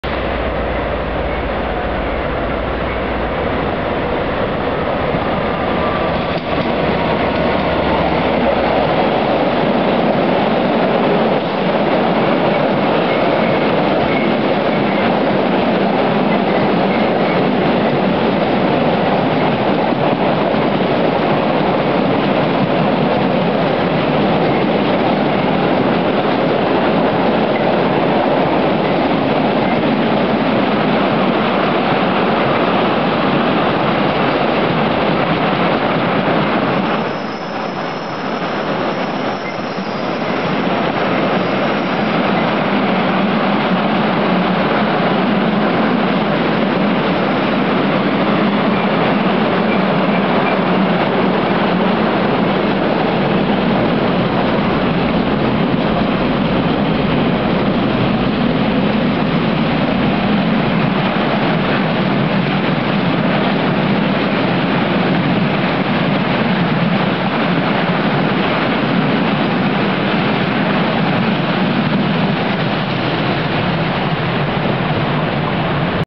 67025 passing Stafford
67025 is seen passing Stafford working 6L48 Garston - Dagenham Empty "Cars"